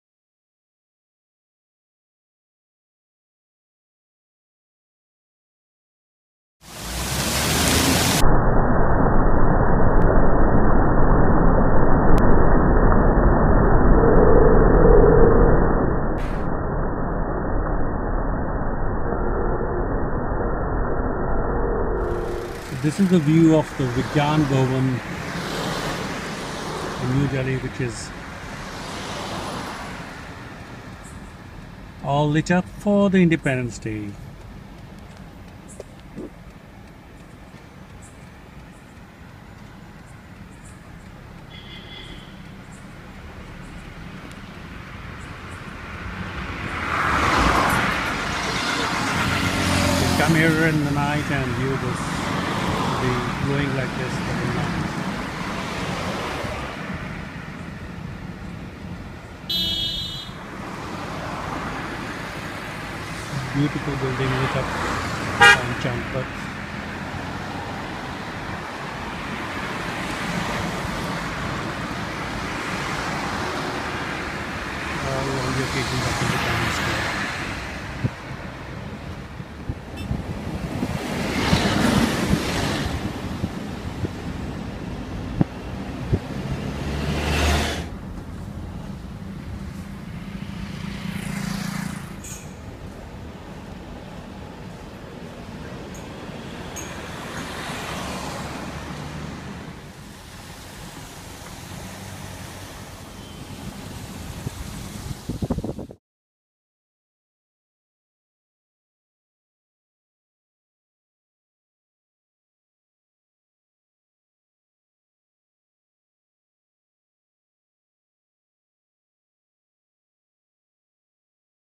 Independence Day Decorations
Beautiful decorations and lighting around Rajpath independenceday2018 Independence Day also makes spectacular views around Rashtrapati Bhawan in New Delhi all government buildings are brightly lit fountains in tri Color and flags all around